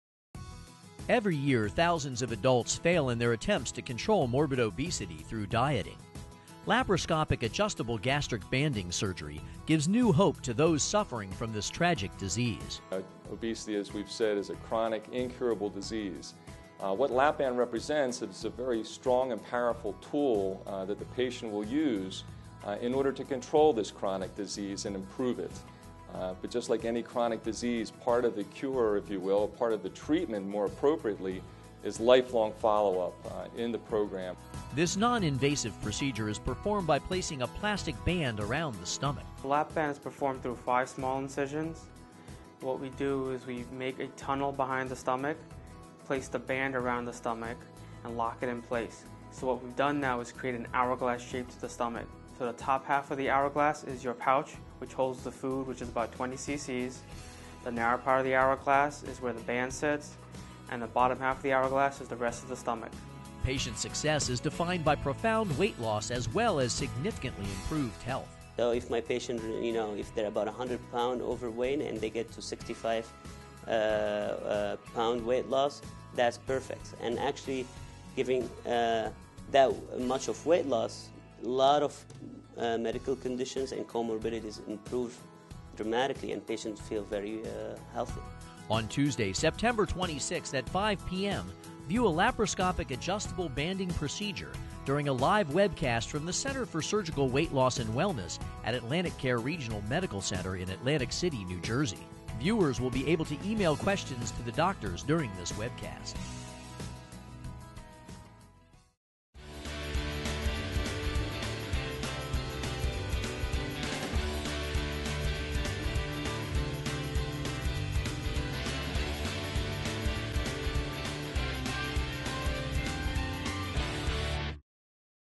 Lap Band Minimally Invasive Weight Loss Surgery
The Center for Surgical Weight Loss & Wellness at AtlantiCare Regional Medical Center (ARMC) will offer the public a chance to watch a laparoscopic adjustable gastric banding weight loss surgery during a live webcast Tuesday, September 26 at 5 p.m.
"This webcast will help the public learn more about this treatment for morbid obesity," said William Aarons, MD, FACS, chairman of the department of surgery at ARMC. "It will also be beneficial for healthcare providers of all levels who want to learn more about this alternative."